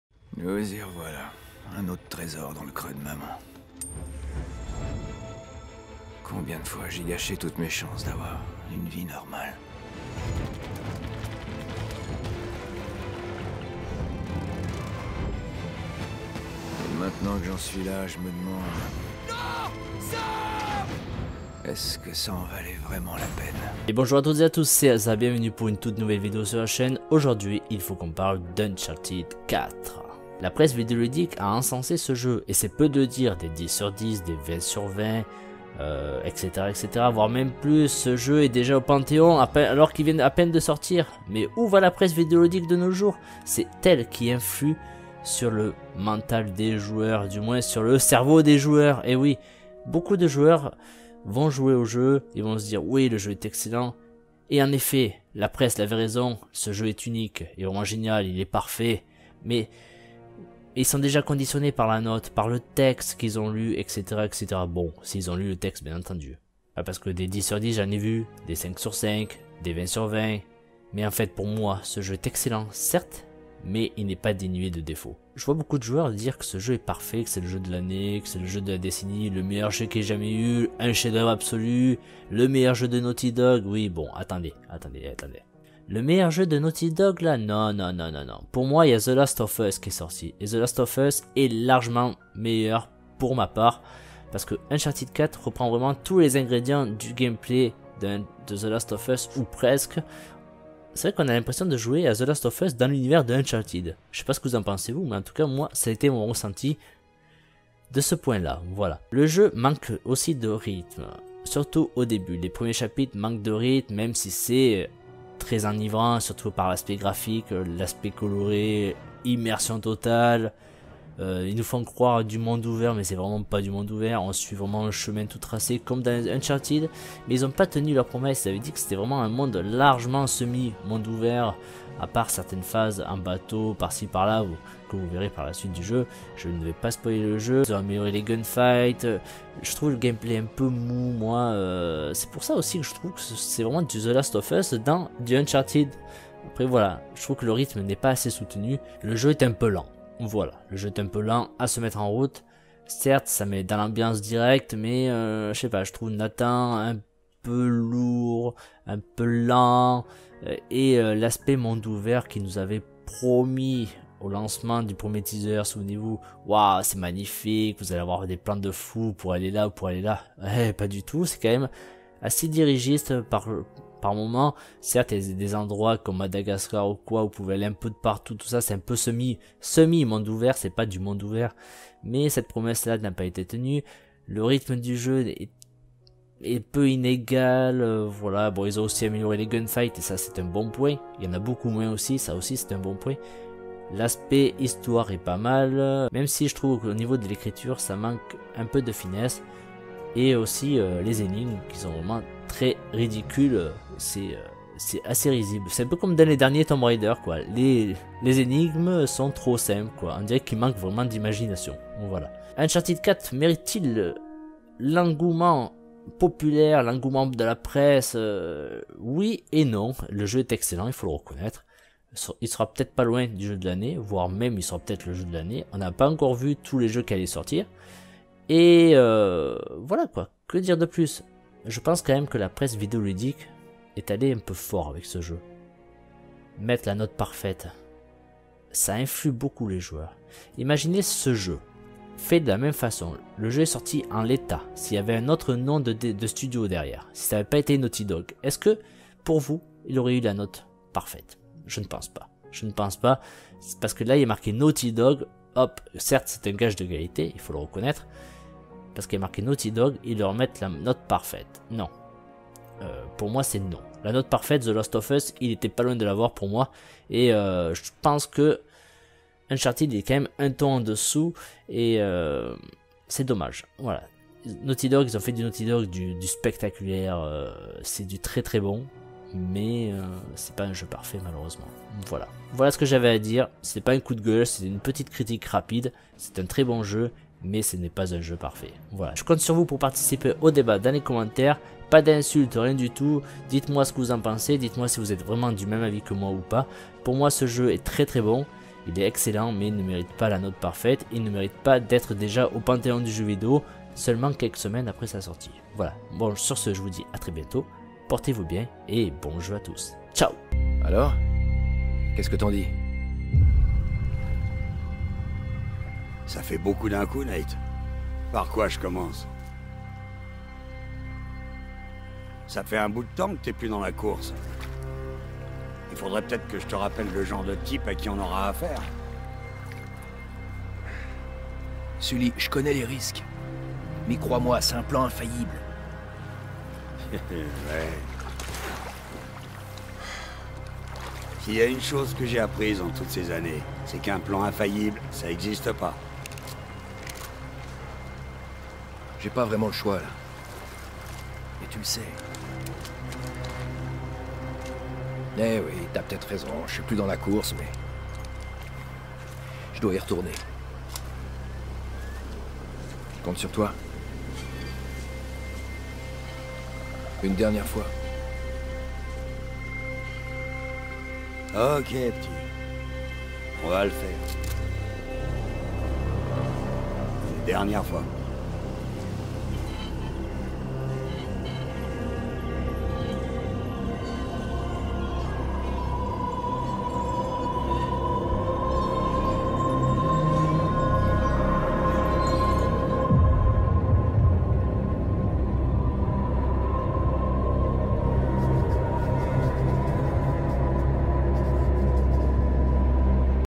Uncharted 4 : A Thief's End - Une aventure CULTE ? Critique FR
Aujourd'hui il faut qu'on parle ! "critique" : Uncharted 4. Voici mon avis sur le jeu, un excellent opus mais. pas parfait.

Partenariat :
► GAMONIAC : 7 € de réduction / Inscription sur l’offre de votre choix à mon code « asajapantvgamo7 »